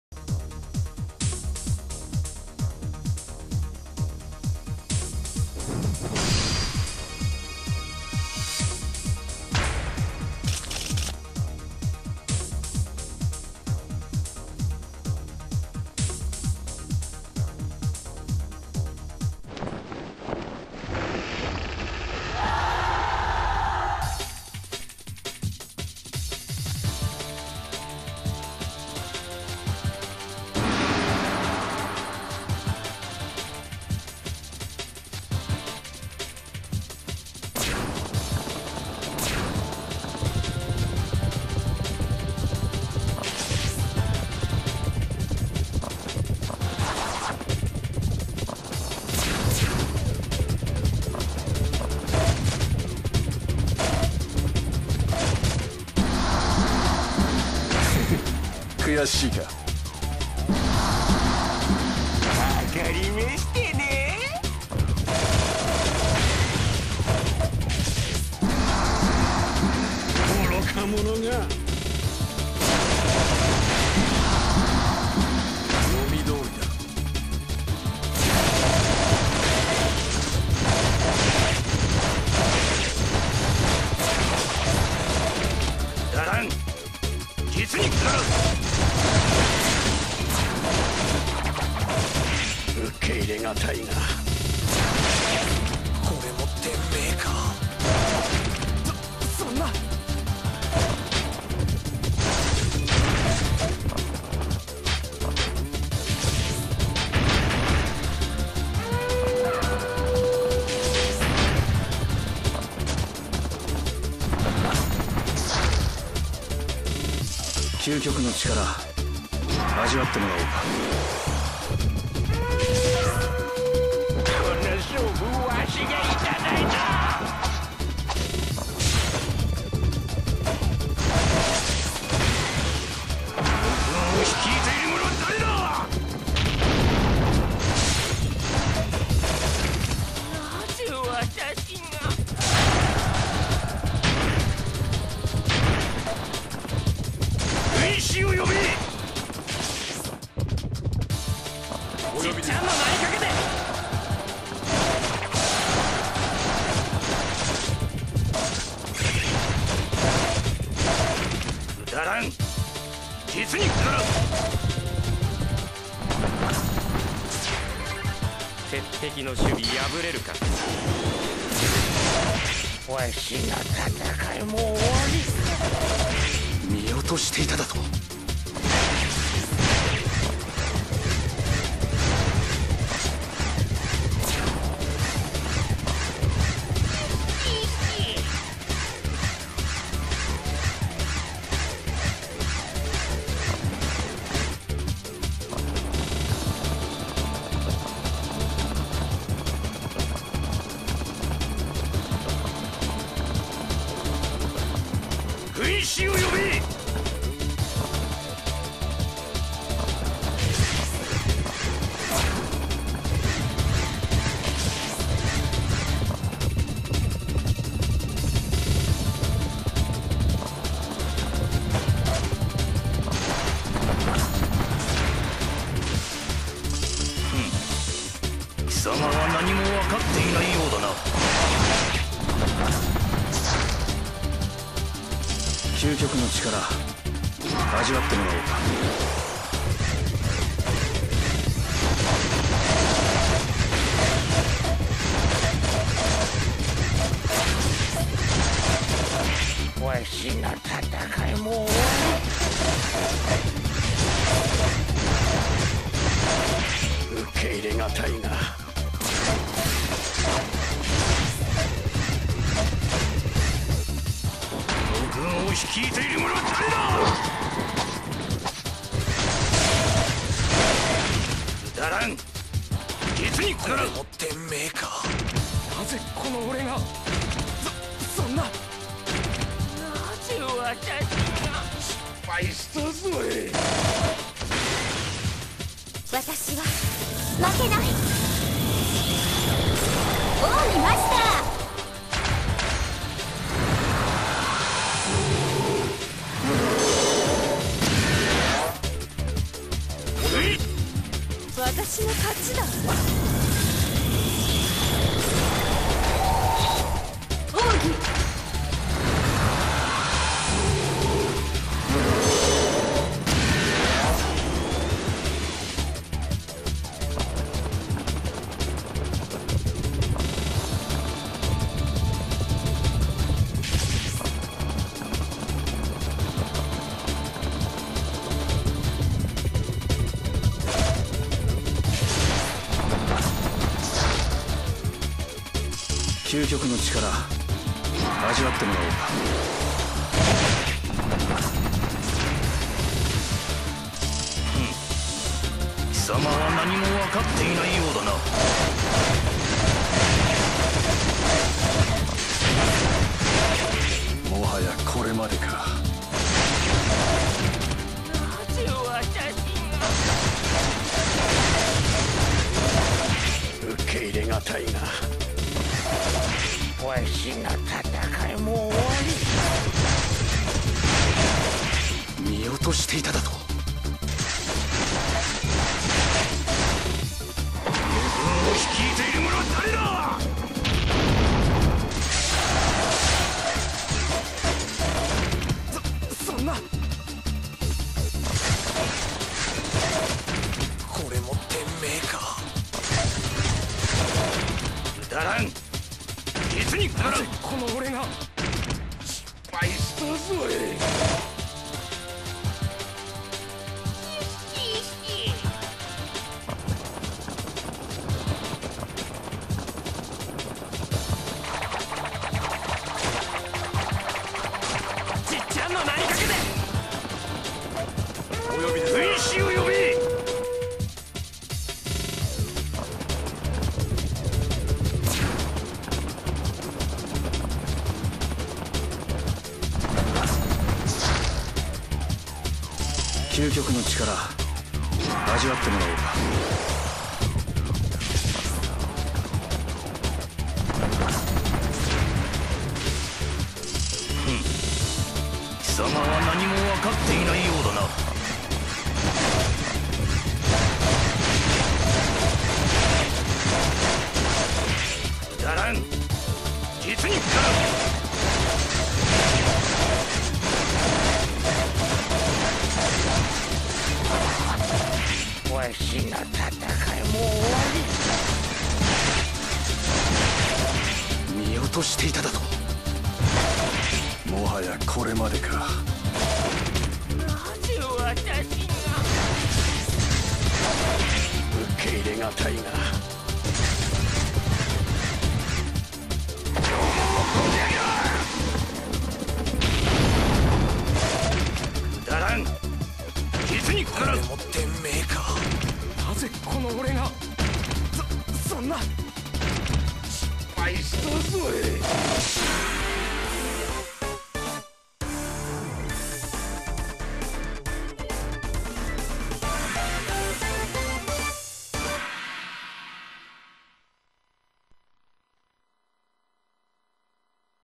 ver 3.59_C_3 演武場 10cost勢力解除大會 -- 至高白馬陣 vs 究極至高
至高白馬陣：SR孫堅、SR諸葛亮、R司馬炎、UC逢紀、UC公孫瓚、C文聘
究極至高：SR孫堅、SR孫策、SR田豐、SR徐庶、SR黃忠